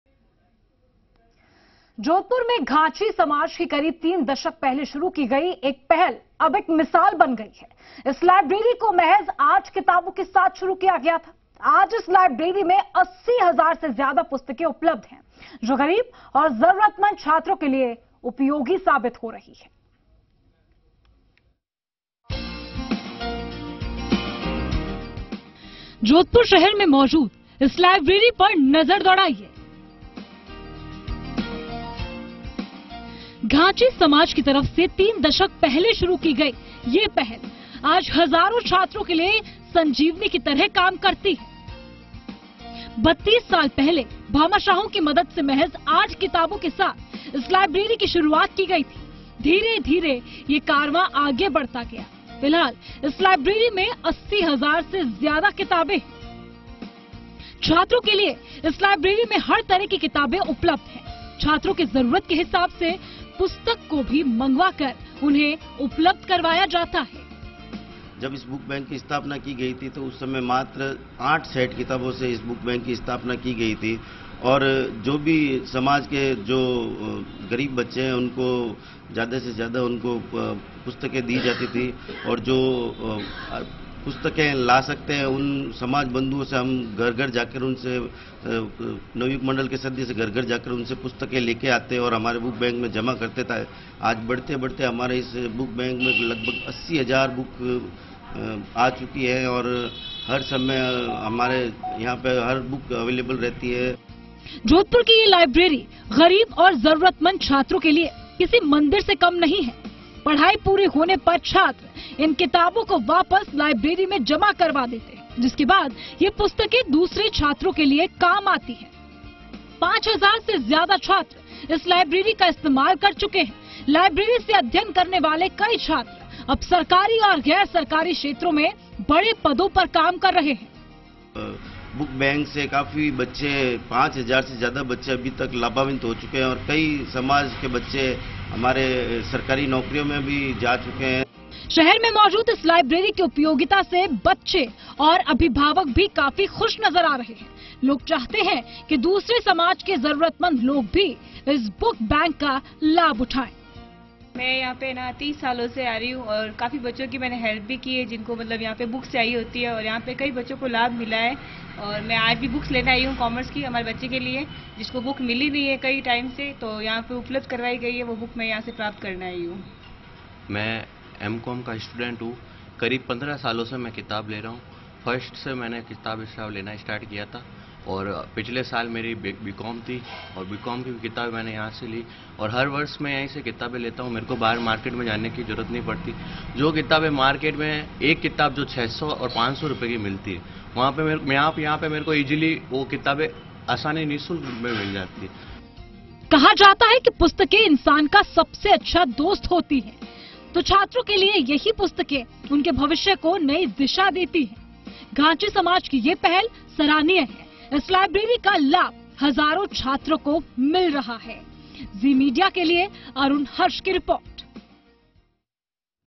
ये लाइब्रेरी गरीब और जरूरतमंद छात्रों के लिए किसी मंदिर से कम नहीं
जोधपुर में घांची समाज की करीब 3 दशक पहले  शुरू की गई एक पहल अब एक मिसाल बन गई है..। इस लाइब्रेरी को महज 8 किताबों के साथ शुरू की गई थी..आज इस लाइब्रेरी में  80 हजार से अधिक पुस्तकें उपलब्ध हैं...जो गरीब और जरूरतमंद छात्रों के लिए उपयोगी साबित हो रहे हैं।